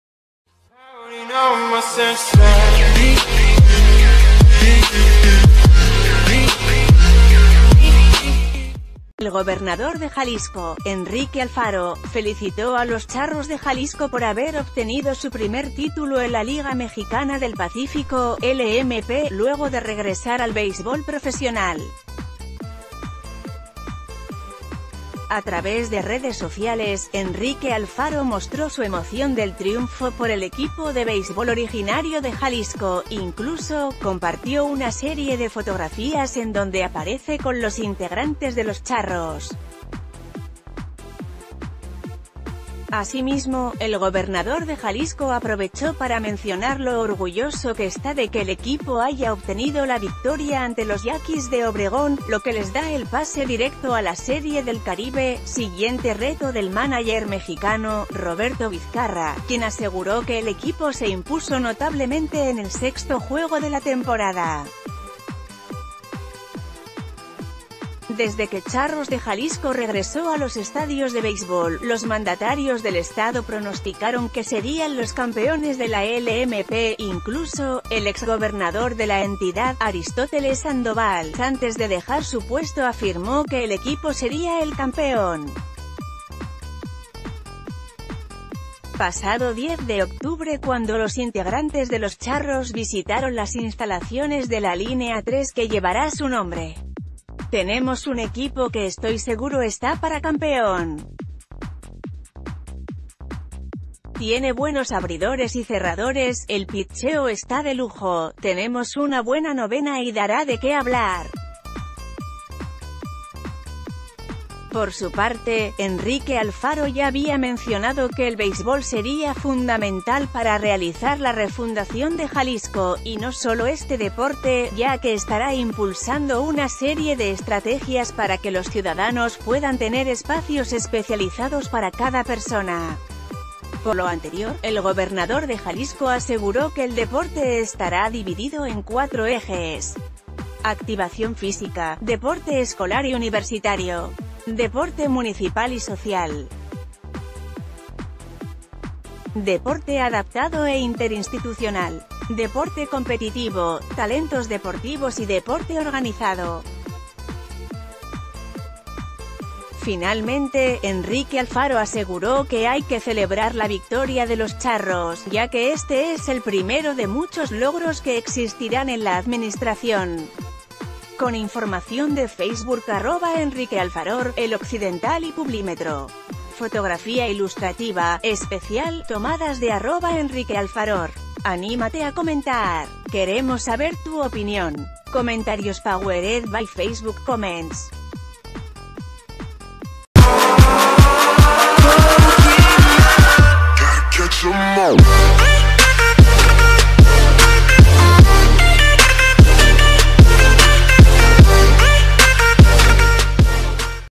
Alfaro felicita a Charros de Jalisco por triunfo en LMP; reitera apoyo al deporte
El gobernador de Jalisco, Enrique Alfaro, felicitó a los Charros de Jalisco por haber obtenido su primer título en la Liga Mexicana del Pacífico (LMP) luego de regresar al beisbol profesional.